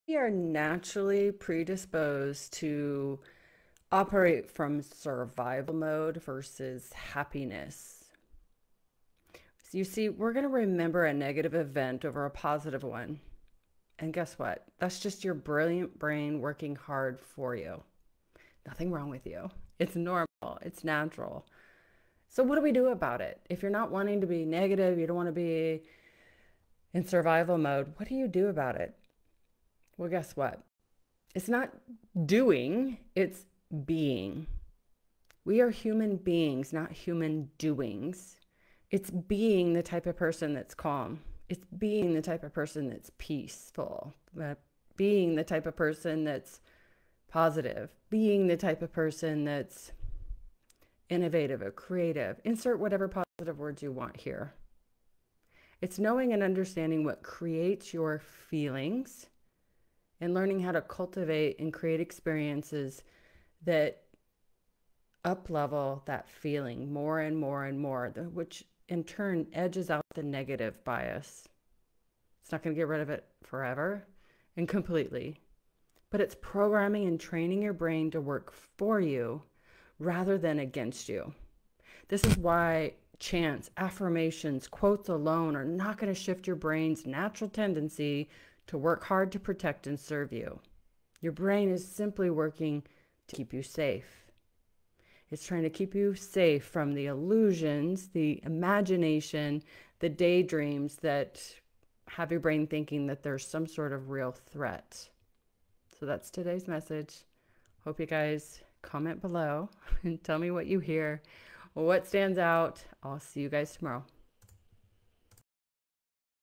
We are predisposed to survival not happiness
We will remember a negative event over a positive one and that’s just your brilliant brain working hard for you…